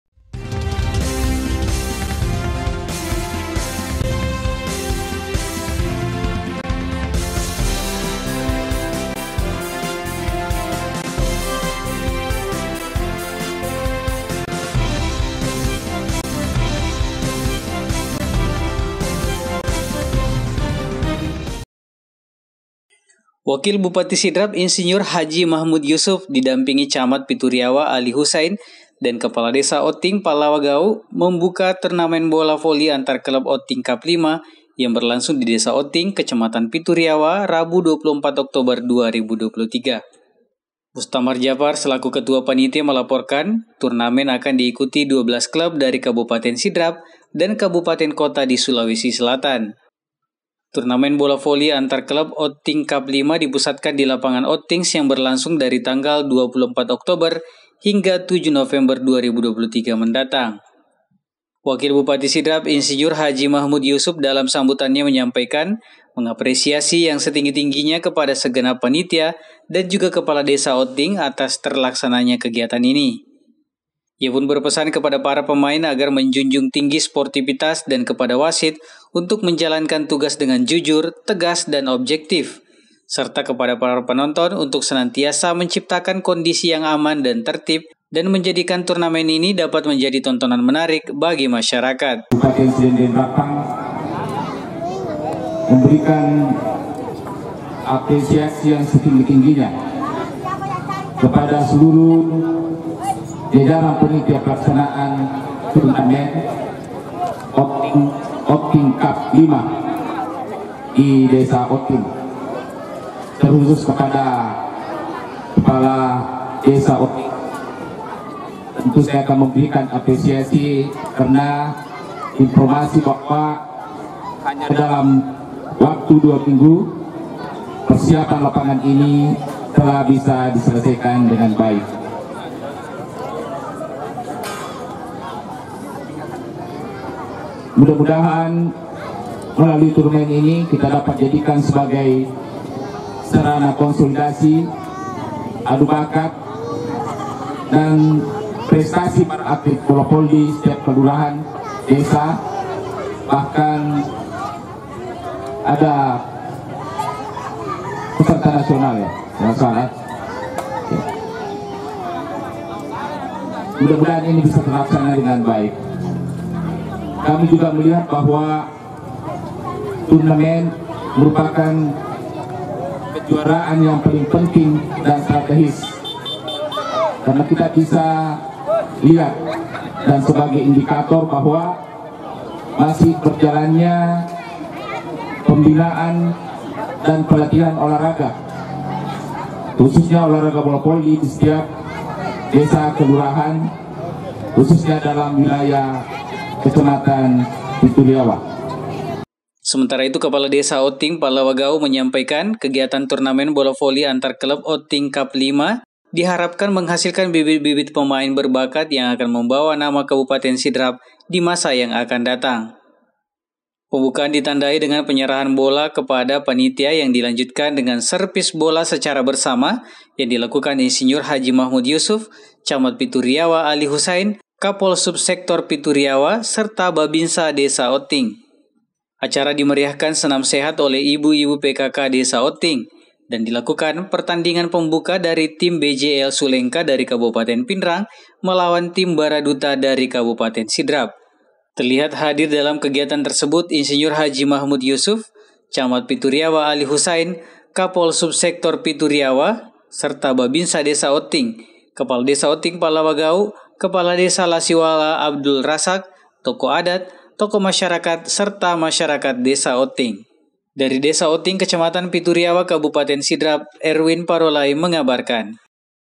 WAKIL BUPATI SIDRAP BUKA TURNAMEN BOLA VOLI ANTAR KLUB OTTING CUP V
WAKIL BUPATI SIDRAP, IR H.MAHMUD YUSUF DI DAMPINGI CAMAT PITU RIAWA ALI HUSAIN DAN KEPALA DESA OTTING PALLAWA GAU  MEMBUKA TURNAMEN BOLA VOLI ANTAR KLUB OTTING CUP V  YANG BERLANGSUNG DI DESA OTTING KECAMATAN PITU RIAWA, RABU 24 OKTOBER 2023

MUSTAMAR JAFAR SELAKU KETUA PANITIA MELAPORKAN TURNAMEN AKAN DIIKUTI 12 KLUB DARI KABUPATEN SIDRAP DAN KABUPATEN KOTA DI SULAWESI SELATAN
 TURNAMEN BOLA VOLI ANTAR KLUB OTTING CUP V  DIPUSATKAN DI LAPANGAN OTTINGS, YANG BERLANGSUNG DARI TANGGAL 24 OKTOBER HINGGA 7 NOVEMBER 2023 MENDATANG.
, WAKIL BUPATI SIDRAP IR H.MAHMUD YUSUF DALAM SAMBUTANNYA MENYAMPAIKAN MENGAPRESIASI YANG SETINGGI TINGGINYA KEPADA SEGENAP PANITIA DAN JGA KEPALA DESA OTTING ATAS TERLAKSANANYA KEGIATAN INI/
IA PUN BERPESAN KEPADA PARA PEMAIN AGAR MENJUNJUNG TINGGI SPORTIFITAS DAN KEPADA WASIT UNTUK MENJELANKAN TUGAS DENGAN JUJUR TEGAS DAN OBJEKTIF SERTA KEPADA PENONTON UNTUK SENANTIASA MENCIPTAKAN KONDISI YANG AMAN DAN TERTIB DAN MENJADIKAN TURNAMENINI DAPAT MENJADI TONTONAN MENARIK BAGI MASYARAKAT.
SEMENTARA ITU KEPALA DESA OTTING PALLA GAU MENYAMPAIKAN KEGIATAN TURNAMEN BOLA VOLI ANTAR KLUB OTTING CUP 5 DIHARAPKAN MENGHASILKAN BIBIT-BIBIT PEMAIN BERBAKAT YANG AKAN MEMBAWA NAMA KABUPATEN SIDRAP DI MASA YANG AKAN DATANG

PEMBUKAAN DITANDAI DENGAN PENYERAHAN BOLA KEPADA PANITIA YANG DI LANJUTKAN DENGAN SERVIS BOLA SECARA BERSAMA YANG DILAKUKAN IR H MAHMUD YUSUF /CAMAT PITU RIAWA ALI HUSAIN/KAPOLSUBSEKTOR PITU RIAWA SERTA BABINSA DESA OTTING
 ACARA DIMERIAHKAN SENAM SEHAT  OLEH IBU-IBU PKK DESA OTTING.DAN DILAKUKAN PERTANDINGAN PEMBUKA DARI TIM BJL SULENGKA DARI KABUPATEN PINRANG MELAWAN TIM BHARADUTA DARI KABUPATEN SIDRAP
TERLIHAT HADIR DALAM KEGIATAN TERSEBUT IR H MAHMUD YUSUF /CAMAT PITU RIAWA ALI HUSAIN/KAPOLSUBSEKTOR PITU RIAWA SERTA BABINSA DESA OTTING/KEPALA DESA OTTING PALLAWA GAU/KEPALA DESA LASIWALA ABDUL RAZAK/TOKOH ADAT/TOKOH MASYRAKAT SERTA MASYARAKAT DESA OTTING